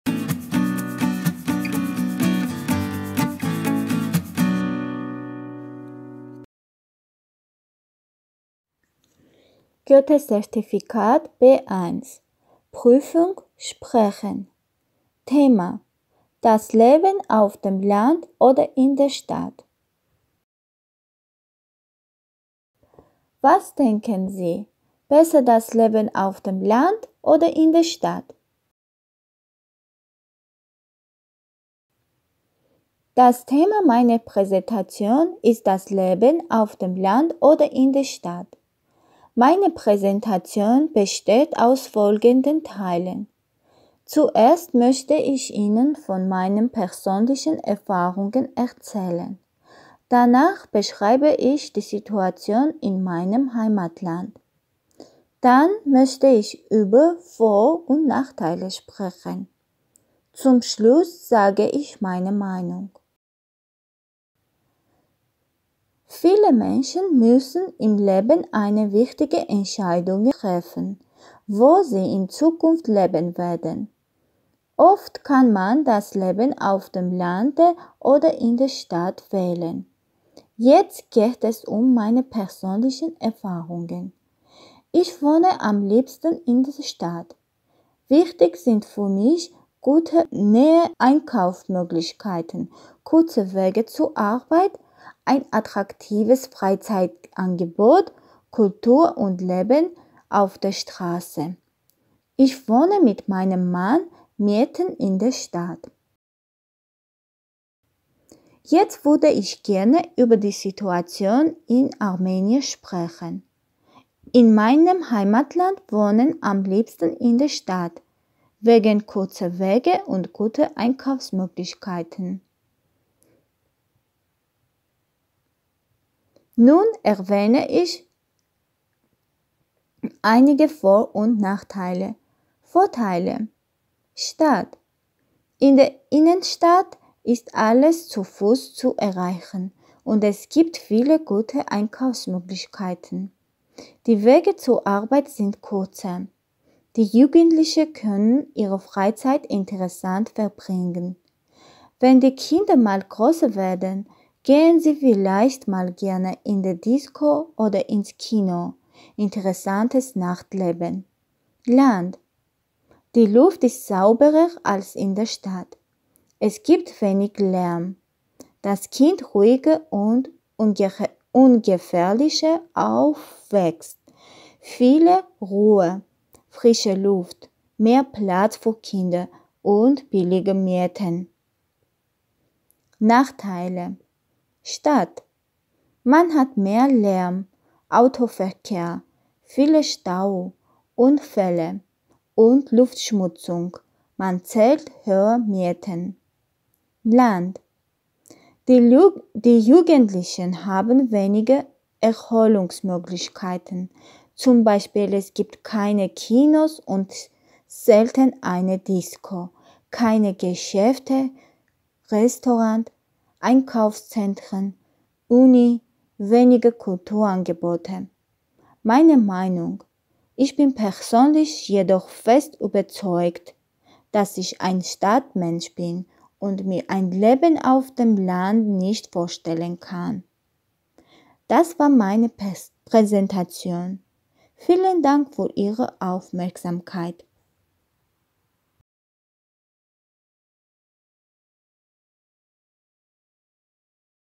leben auf dem land oder in der stadt vor- und nachteile
Das Leben auf dem Land oder in der Stadt (Was denken Sie besser, das Leben auf dem Land oder in der Stadt?)
Das Thema meiner Präsentation ist das Leben auf dem Land oder in der Stadt. Meine präsentation besteht aus Folgenden Teilen: Zuerst möchte ich Ihnen von meinen persönlichen Erfahrungen erzählen. Danach beschreibe ich die Situation in meinem Heimatland. Dann möchte ich über Vor- und Nachteile sprechen. Zum Schluss sage ich meine Meinung.
 Viele Menschen müssen im Leben eine wichtige Entscheidung treffen, wo sie in Zukunft leben werden. Oft kann man das Leben auf dem Lande oder in der Stadt wählen. Jetzt geht es um meine persönlichen Erfahrungen. Ich wohne am liebsten in der Stadt. Wichtig sind für mich gute und nahe Einkaufsmöglichkeiten, kurze Wege zur Arbeit, ein attraktives Freizeitangebot, Kultur und Leben auf der Straße. Ich wohne mit meinem Mann mitten in der Stadt.
Jetz würde ich gerne über die Situation im Armenier sprechen. In meinem Heimatland wohnen am libsten in der Stadt wegen kurzer Wege und guter Einkaufsmöglichkeiten
Nun erwähne ich einige Vor- und Nachteile.
 Vorteile:
Stadt: In der Innenstadt ist alles zu Fuß zu erreichen und es gibt viele gute Einkaufsmöglichkeiten. Die Wege zur Arbeit sind kürzer. Die Jugendliche können ihre Freizeit interessant verbringen. Wenn die Kinder mal grösser werden, gehen sie vielleicht mal gerne in die Disco oder ins Kino. interessantes Nachtleben.
Land: die Luft ist sauberer als in der Stadt. Es gibt weing Lärm. das Kind ruhiger und ungefährlicher aufwächst. viel Ruhe, frische Luft, mehr Plazt für Kinder und billiger Mieten.

Nachteil:
Stadt: man hat mehr Lärm, Autoverkehr, viele Staue, Unfälle und Luftverschmutzung man zählt hohe Mieten.
Land: Die Jugendlichen haben wenige Erholungsmöglichkeiten. z.B. Es gibt keine Kinos und Selten eine Disko, keine Geschäfte, Restaurant, Einkaufszentren, Uni, weniger Kulturangebote. Meiner Meinung: Ich bin persönlich jedoch fest überzeugt, dass ich ein Stadt mensch bin und mir ein Leben auf dem Lande nicht vorstellen kann. Das war meine Präsentation. Vielen Dank für Ihre Aufmerksamkeit. ‫